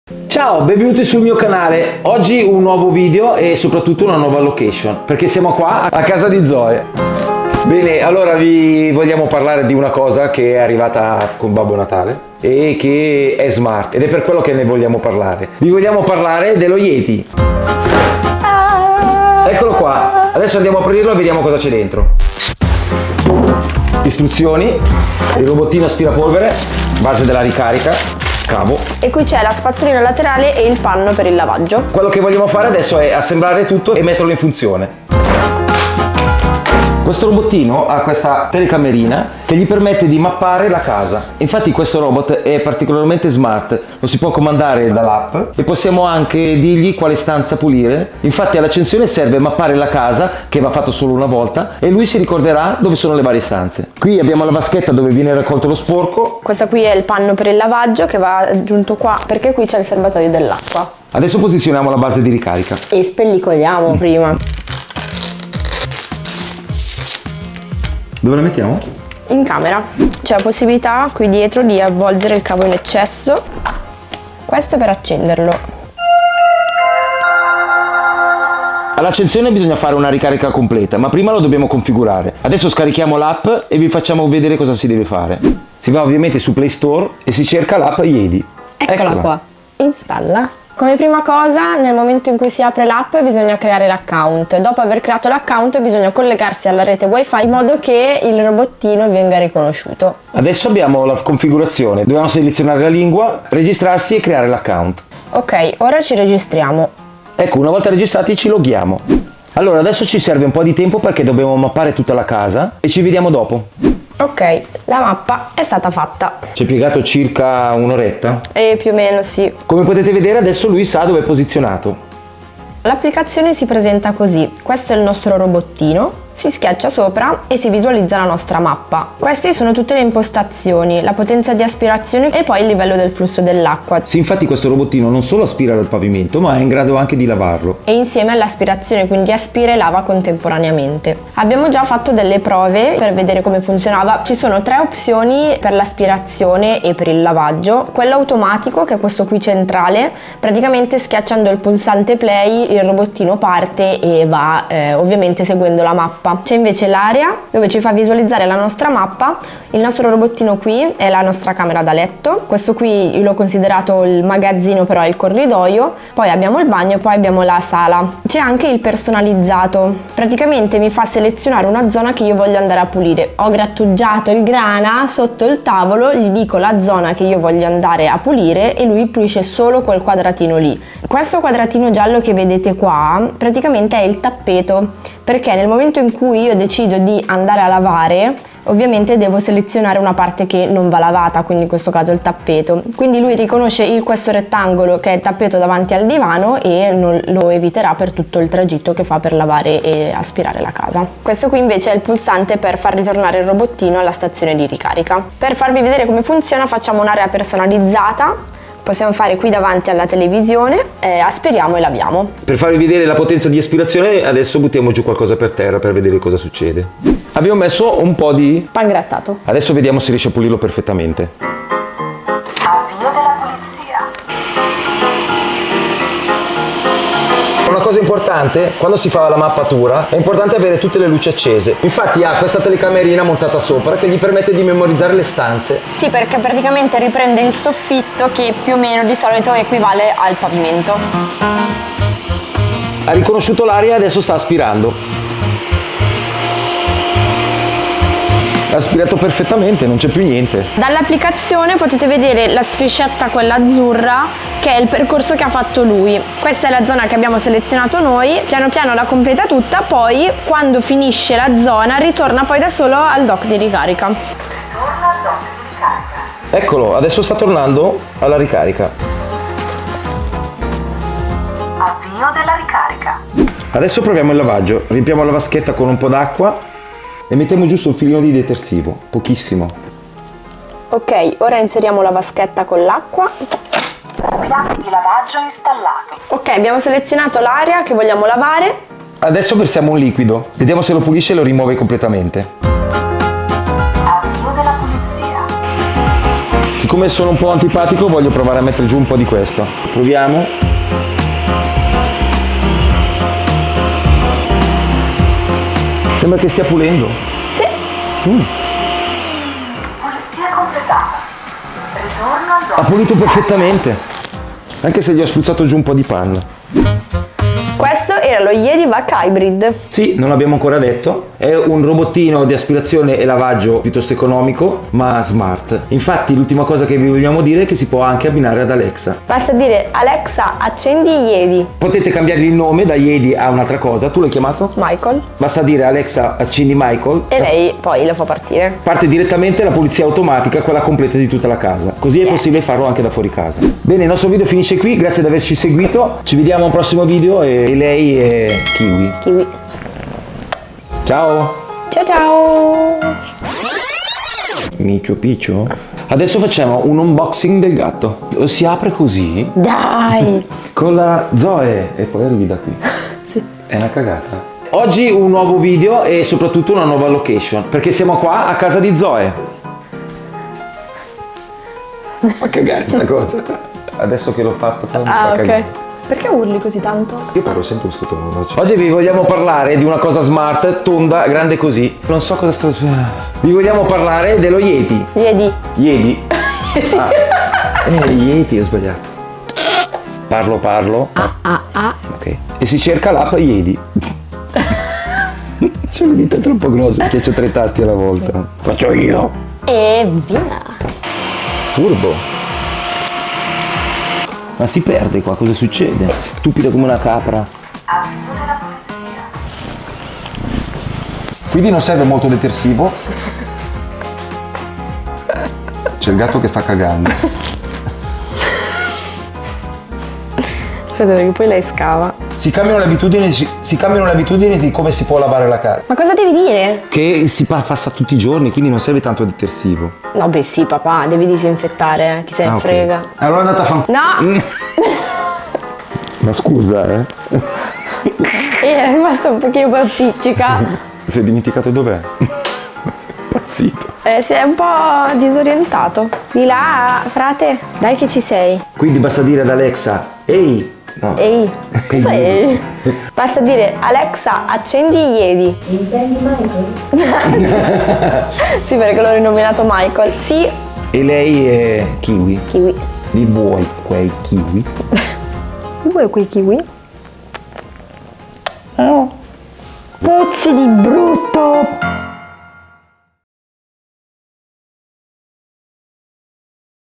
Yeedi vac hybrid Robot - Unboxing e test con Zoe | Robot che lava e aspira economico | Euro Gala
Ci piacciono le cose smart.
Lo Yeedi Robot è un simpatico robot lava aspira pavimenti.
Lo abbiamo testato e messo alla prova con dello sporco.
Funziona con Alexa e con app
Ottimo regalo arrivato a Natale.

Yeedi vac hybrid Robot Aspirapolvere Lavapavimenti, Aspirazione Potenza 2500Pa, rilevamento tappeto, Mappatura di navigazione intelligente, Controllo APP & Alexa